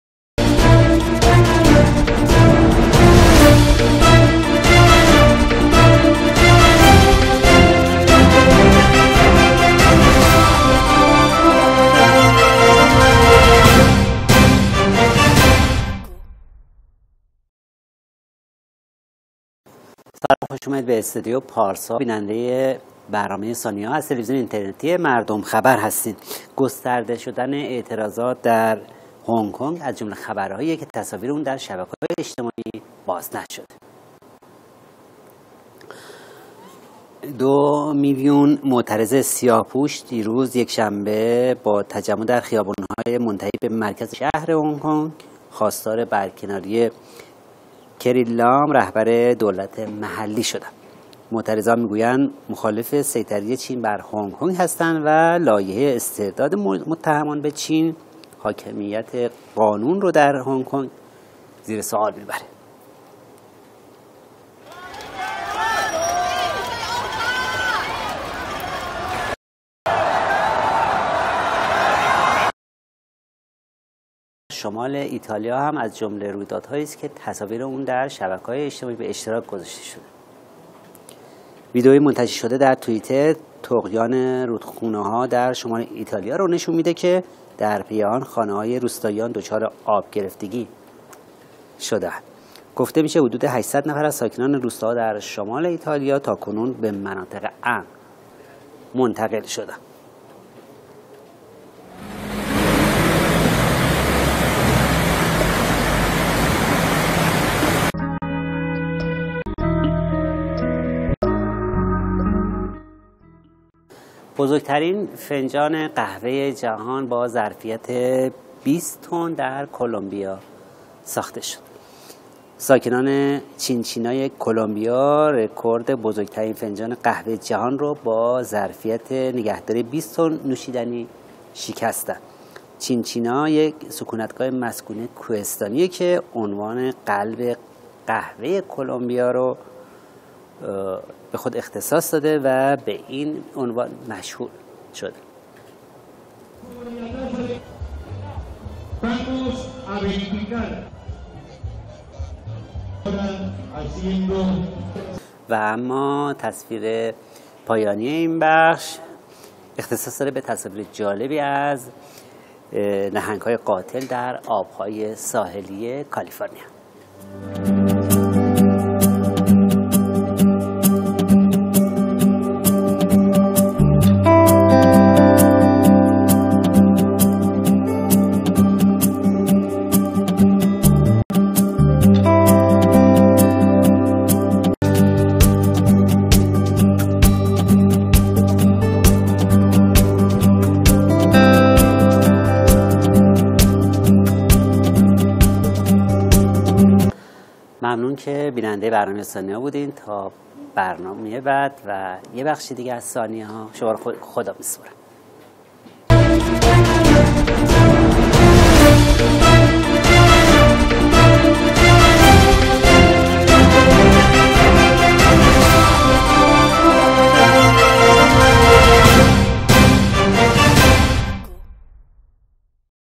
تظاهرات دو میلیونی سياه پوشان چيني برای برکناری رهبر دولت محلي
برای نصب اپلیکیشن مردم خبر روی لینک بزنید